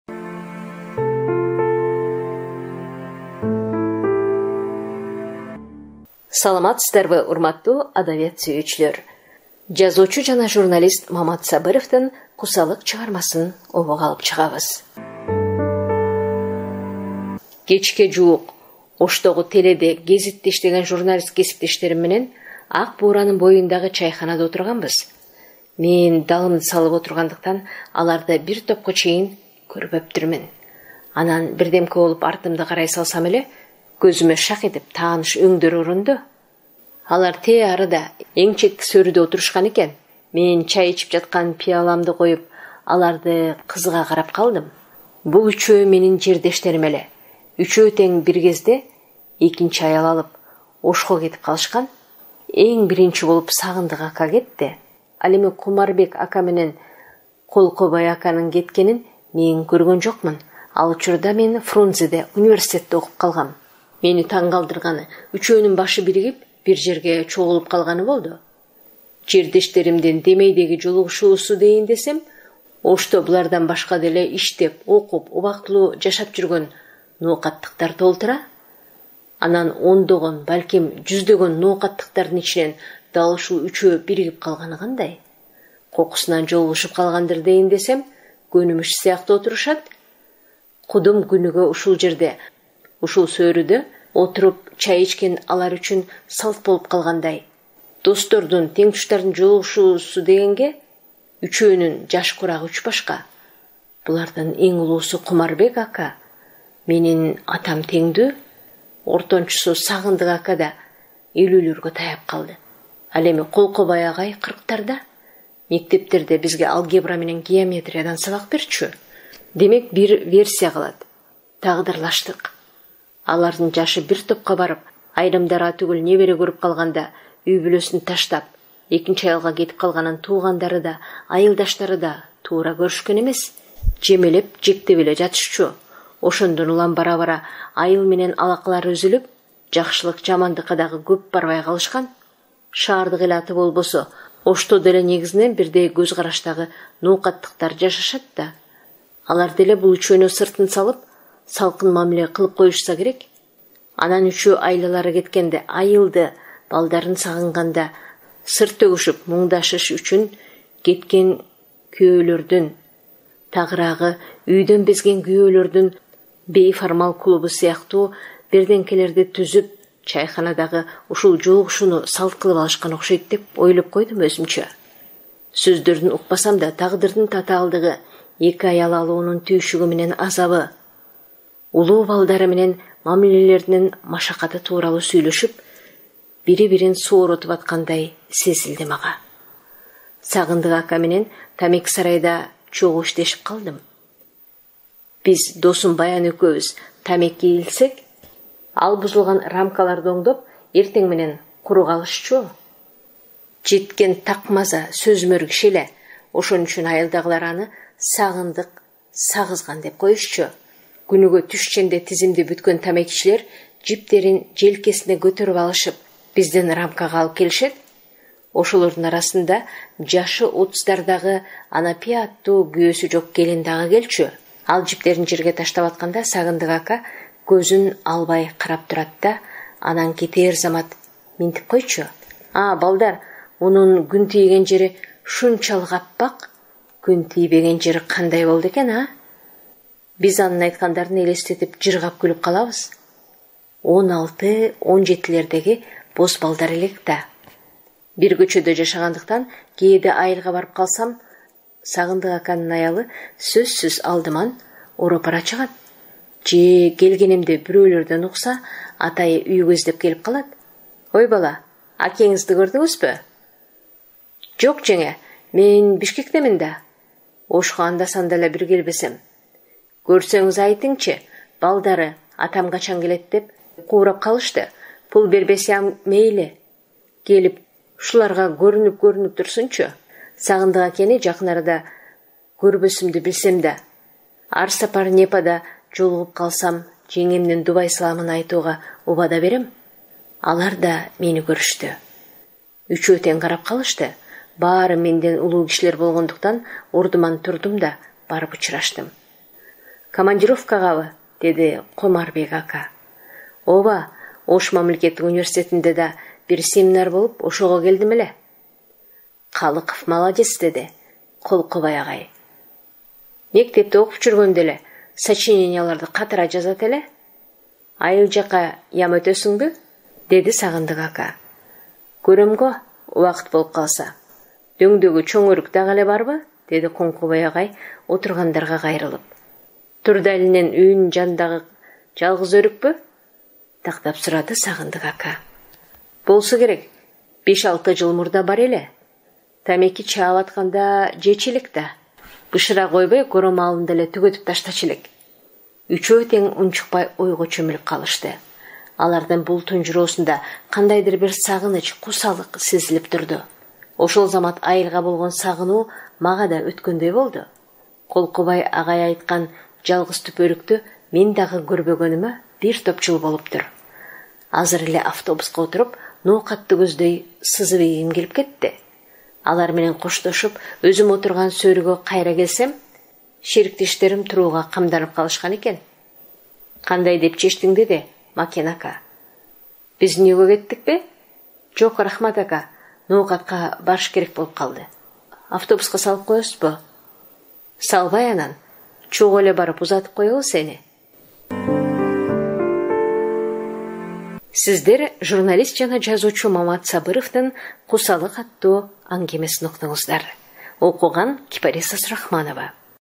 КУСАЛЫК /аңгеме/Мамат Сабыров
Чыгармада бүлө баркын билбеген "билермандар"  баяндалат.

Чыгарманы жазган:
МАМАТ САБЫРОВ
Окуп, аудиолоштурган
КИПАРИСА СУРАХМАНОВА

***АУДИОКИТЕП автордук  укук менен корголот***